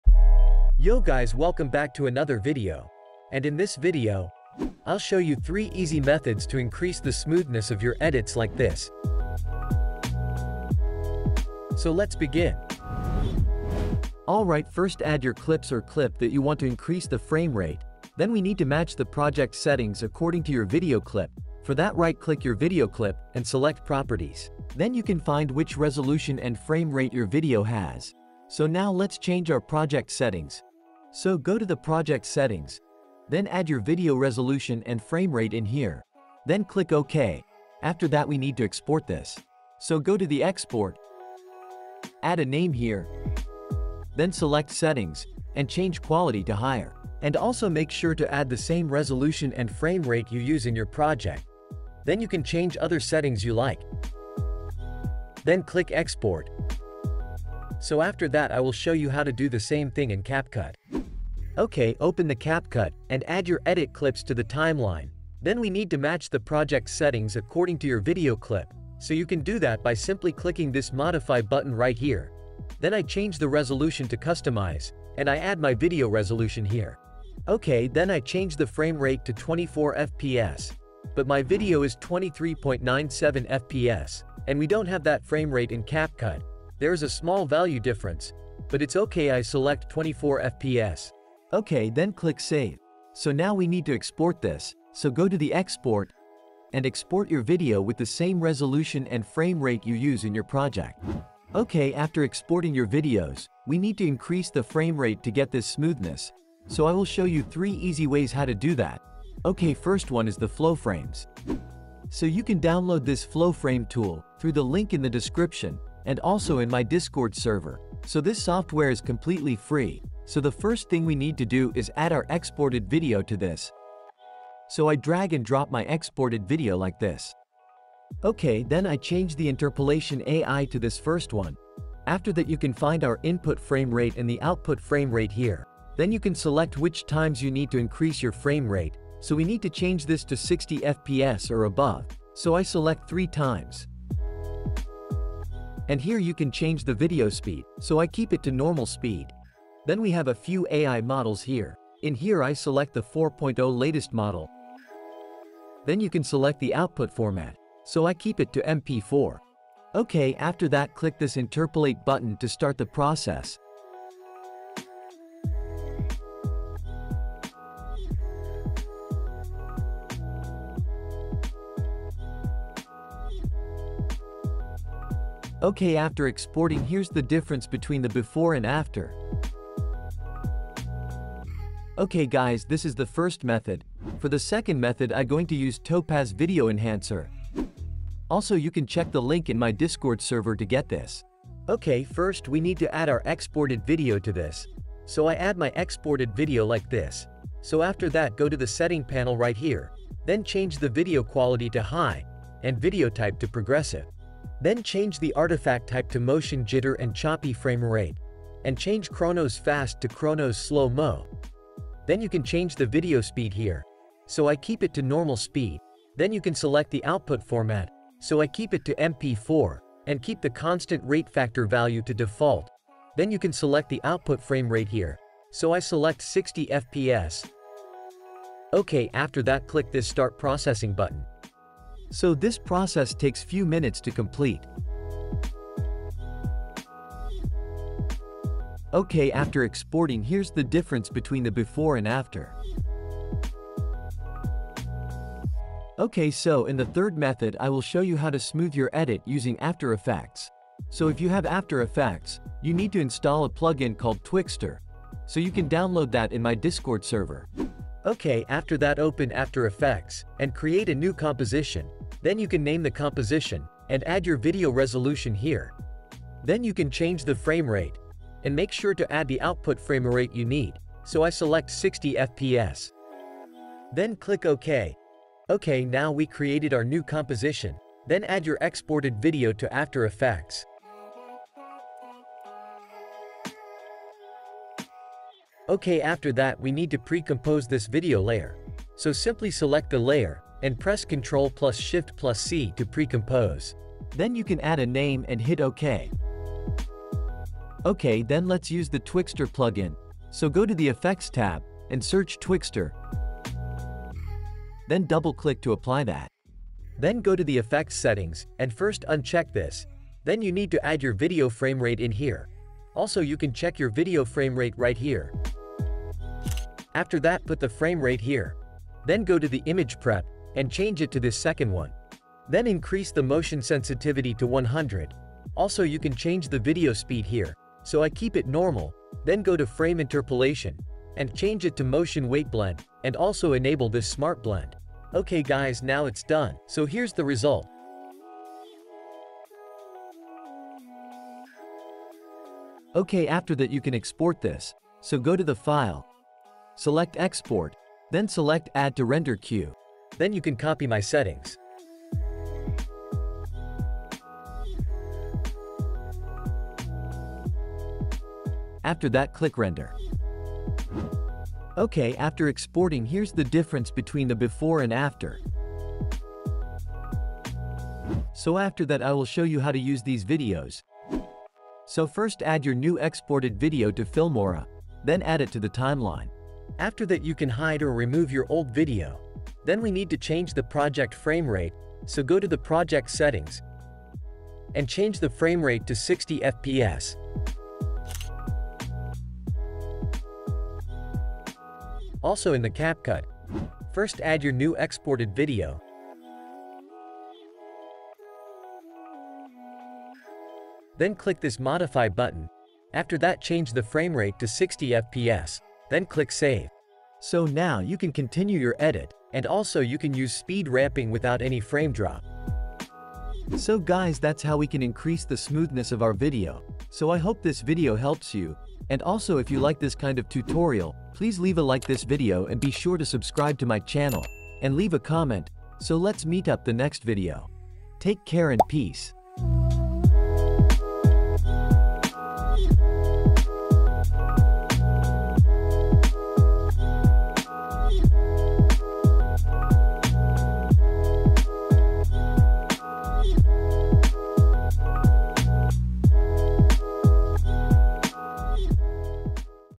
3 EASY ways to Make Your EDITS SMOOTH
Hey everyone! In this video, I'll show you how to Make Your EDITS SMOOTH using three simple methods. We'll be using Flowframes, Topaz Video Enhancer, and Twixtor in After Effects. 😄

💠Video Include:
Smooth Edit Tutorial
Flowframes Tutorial
Topaz Tutorial
Twixtor Tutorial
After Effects Tutorial
how to increase frame rate